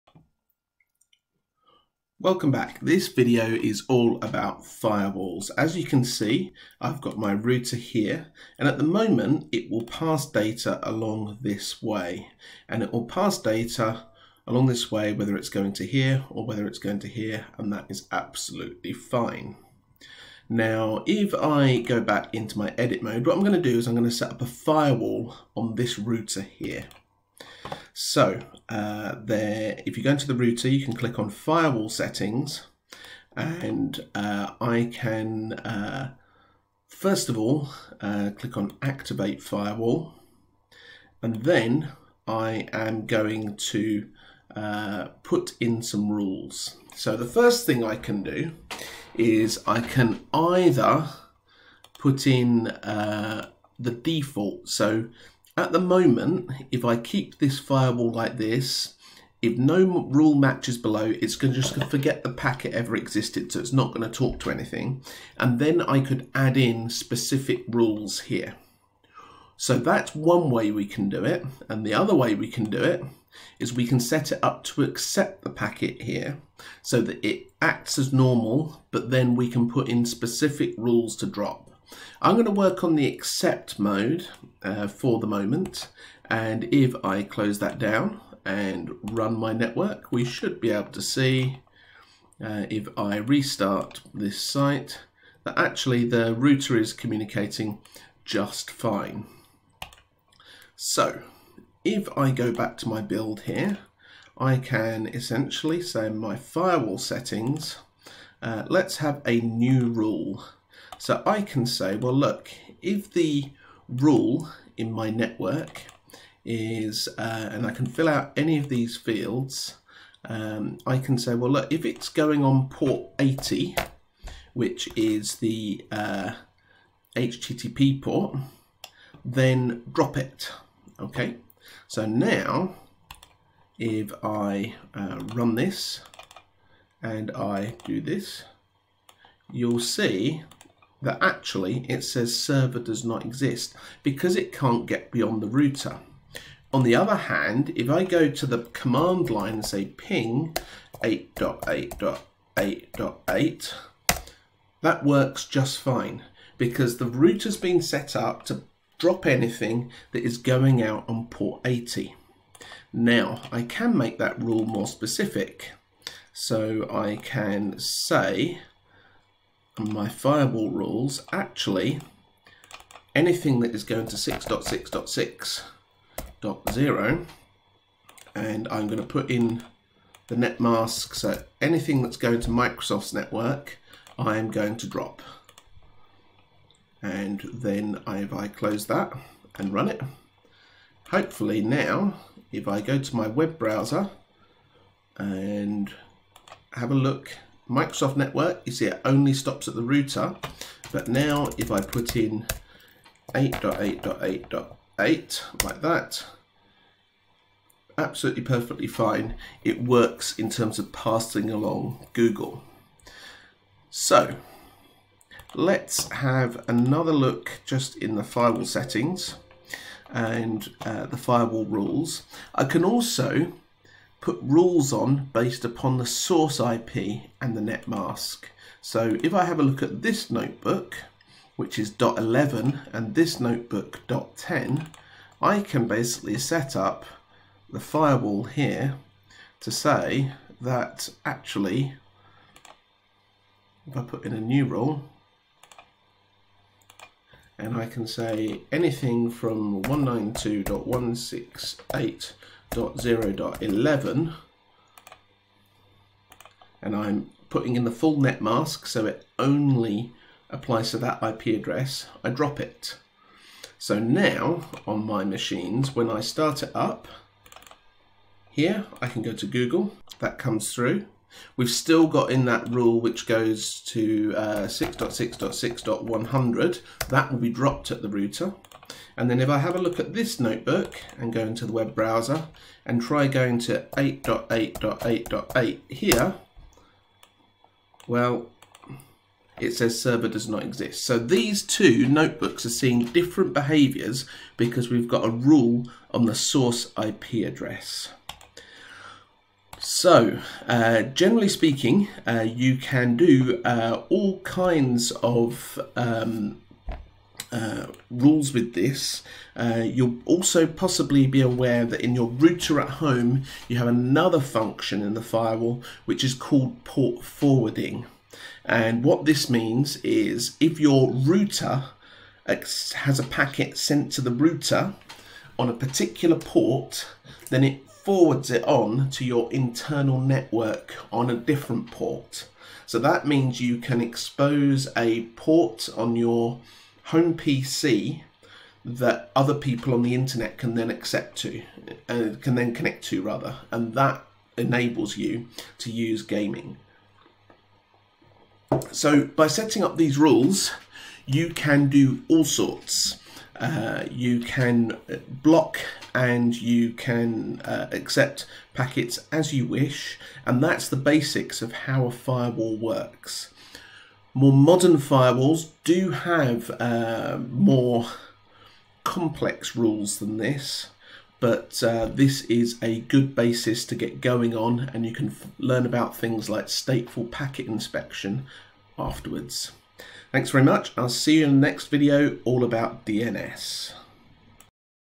Filius - Firewalls
Use filius to set up a basic firewall based upon packet forwarding rules